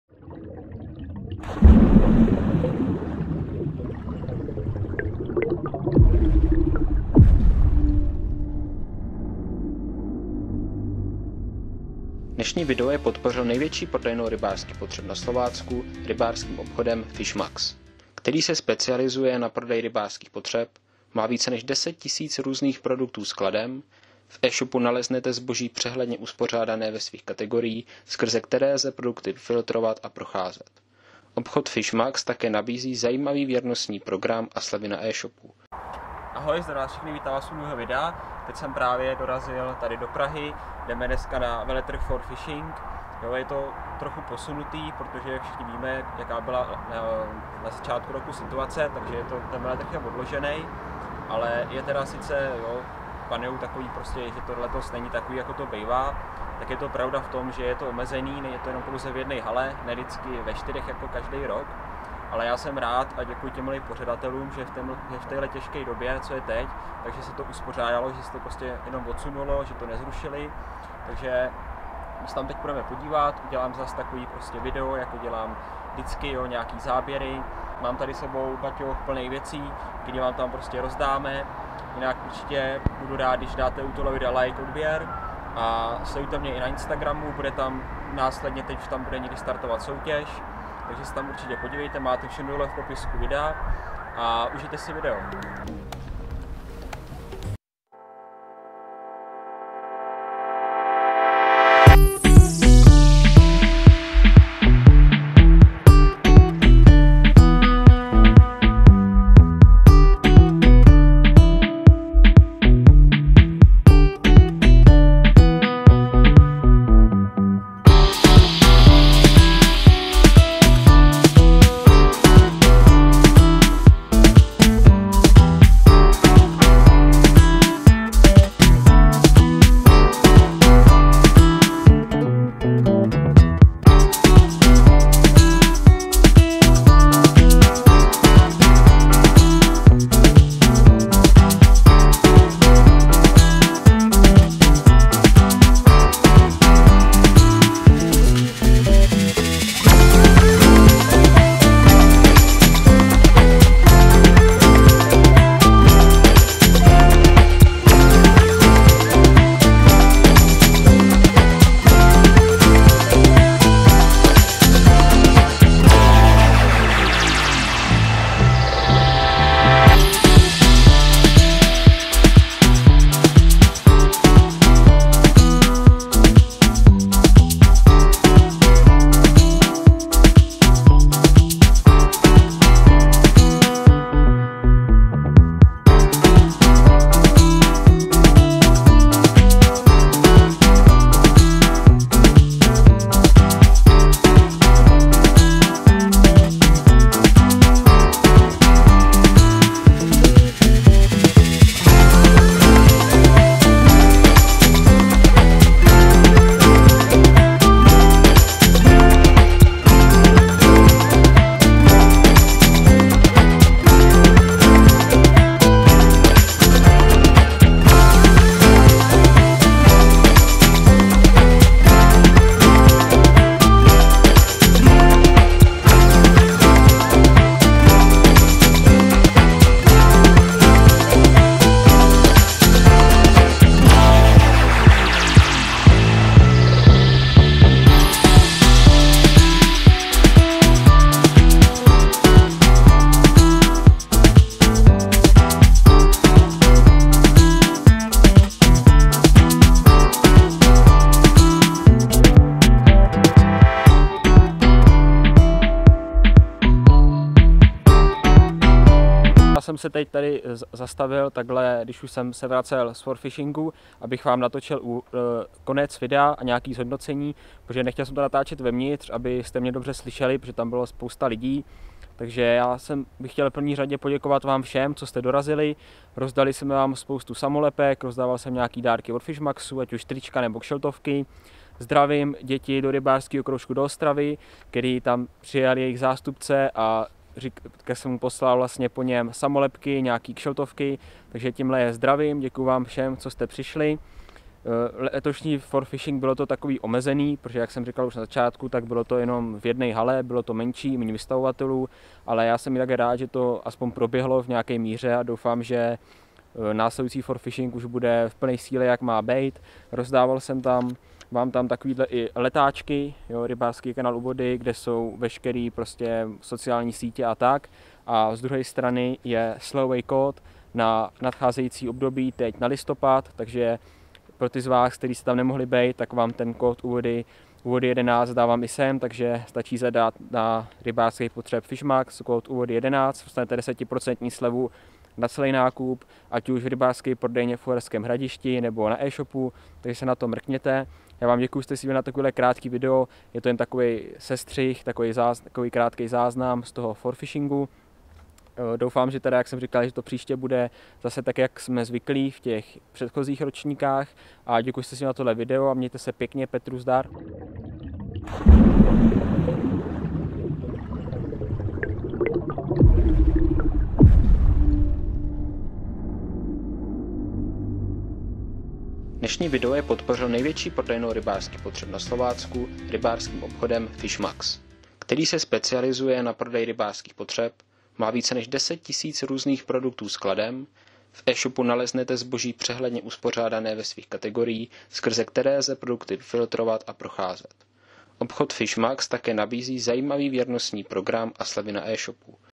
Rybářský veletrh FOR FISHING 2021| Rozdali jsme vám dárky + sleva na váš nákup
Jako každý rok tak i tentokrát jsem se vydal na rybářský veletrh For Fishing v Praze. Kde jsem vám rozdával samolepky a dárky od Fishmax.